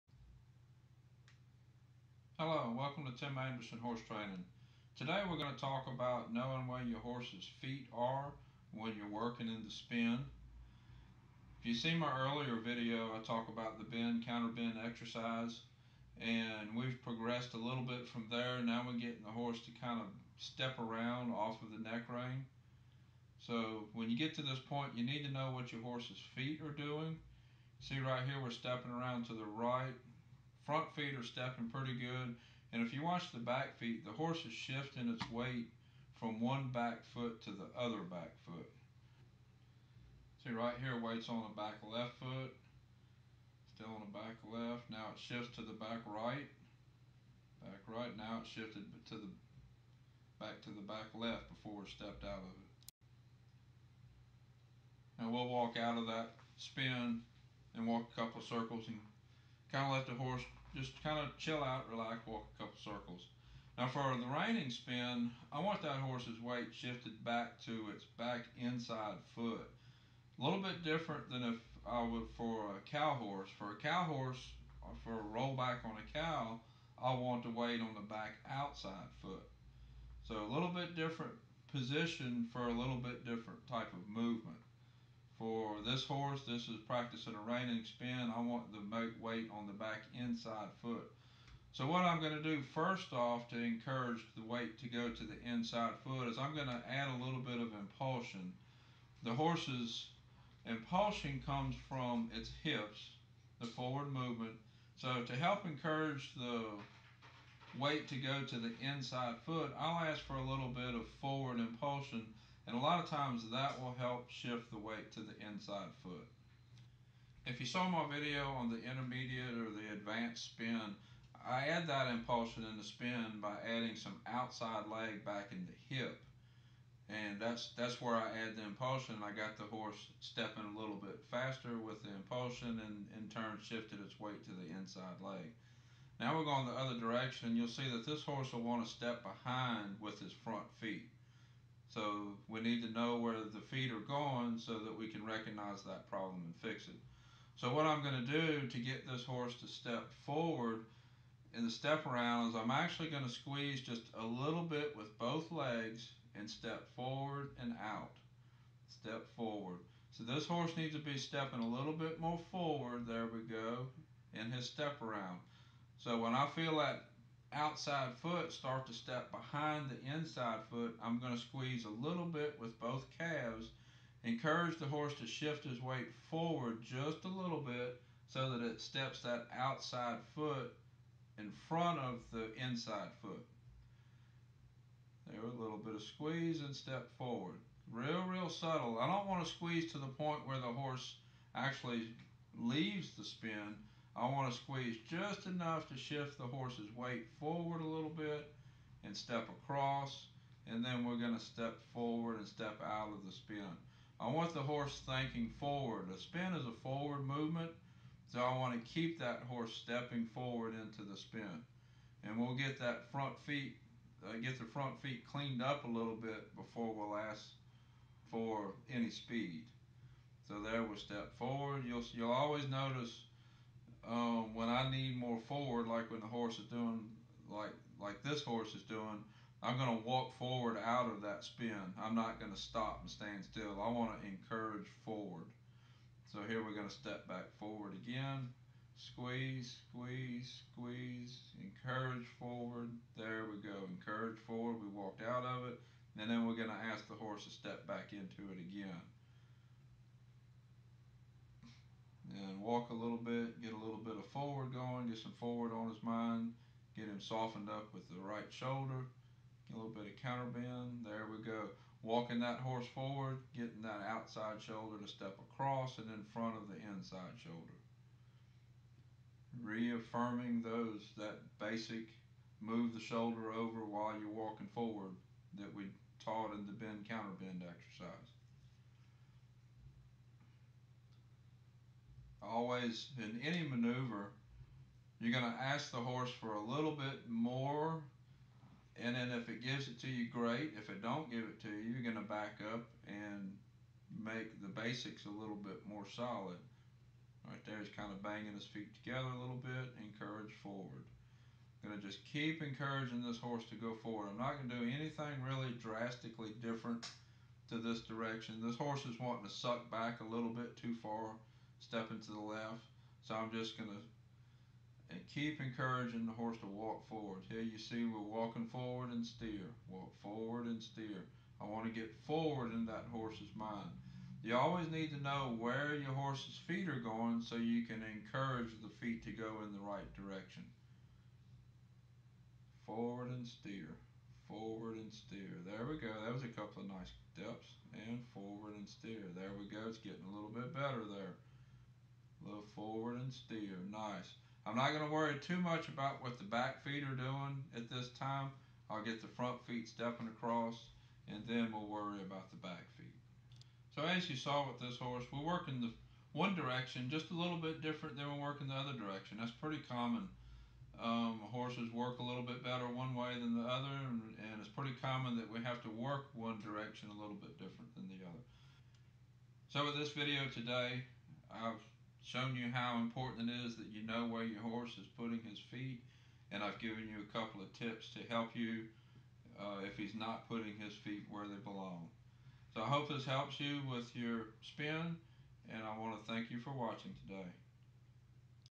Correct Foot Fall And Placement When Training the Reining Spin
In this video I am talking about knowing where your horse's feet are and putting them in the right place when training a horse to do a reining spin.